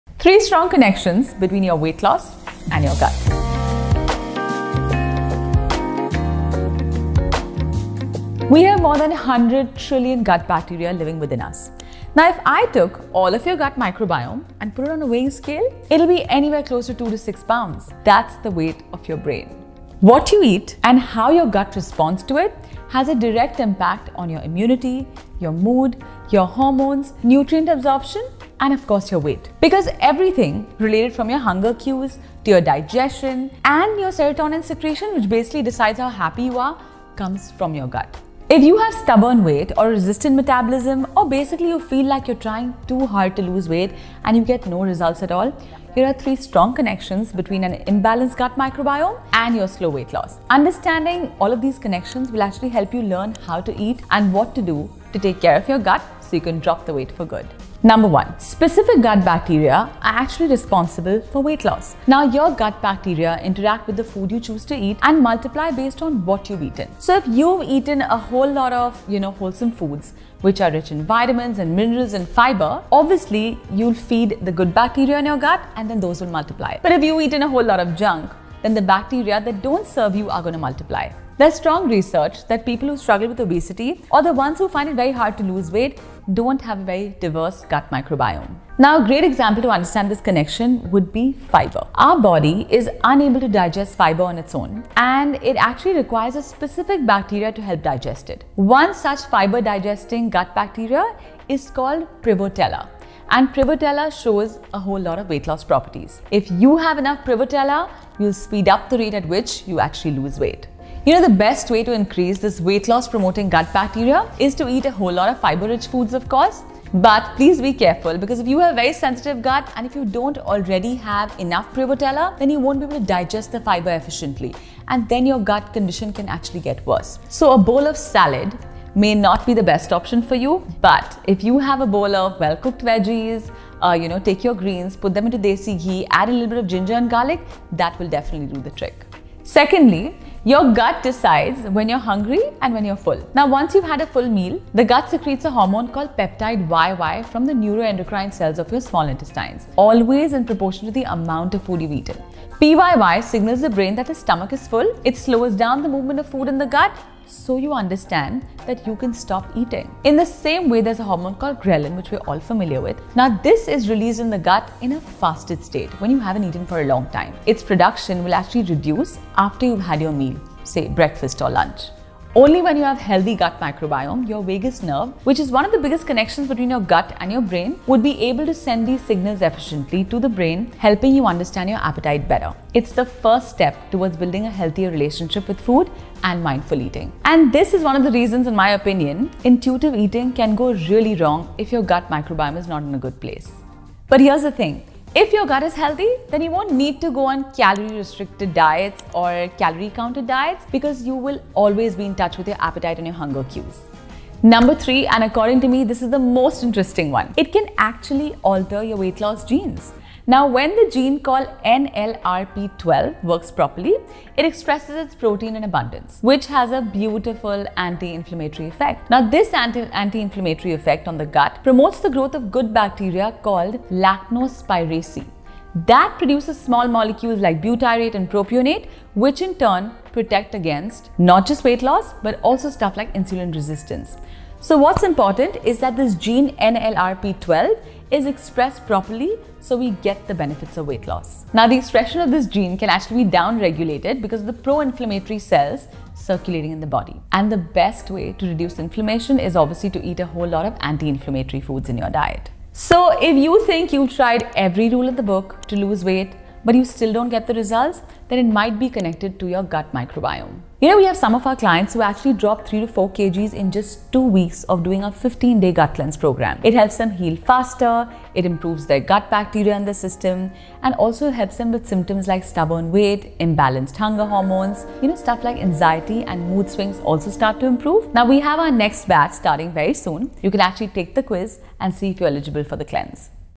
3 Strong Connections between your Weight Loss and your Gut
If you think you’ve tried every rule in the book to lose weight, but still don’t get results, then it might be connected to your gut microbiome.

We have some of our clients who drop 3 kgs in 2 weeks of doing the 15-day Gut Cleanse Program. It helps to heal and restore your gut bacteria so that your symptoms like stubborn weight, imbalanced hunger hormones and even anxiety/mood swings start to improve.